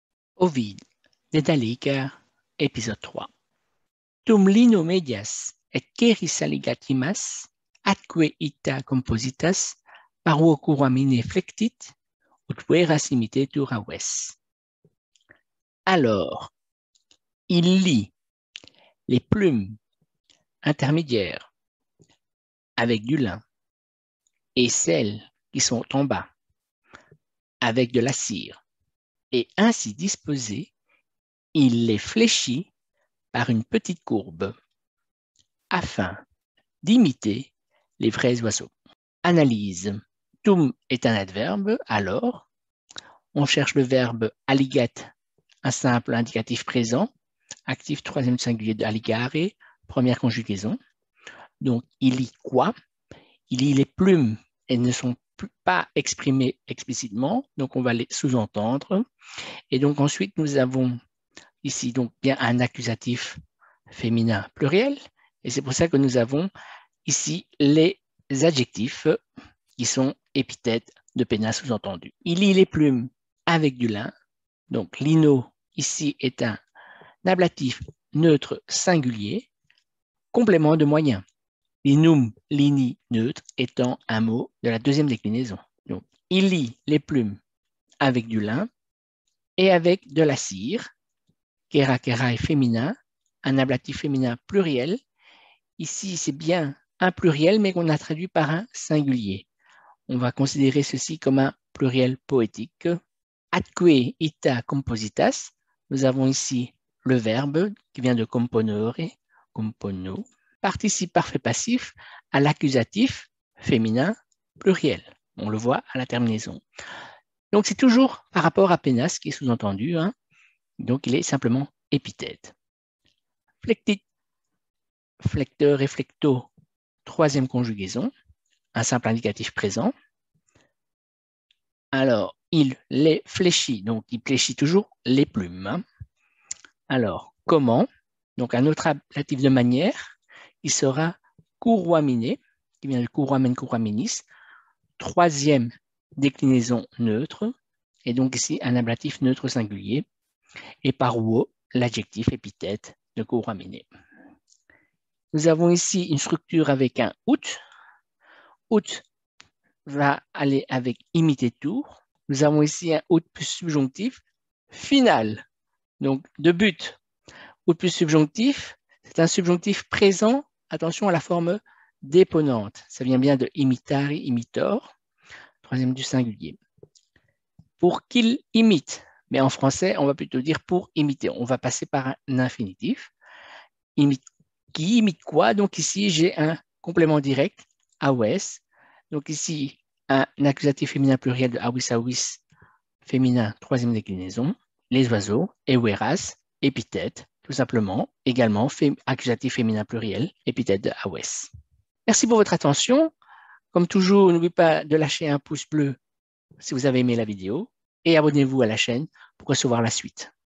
Ovide - Dédale et Icare - Épisode 3 (traduction) - v. 193-195
Traduction littérale et analyse grammaticale du texte d'Ovide, "Les Métamorphoses" (8, 193-195).
Dédale et Icare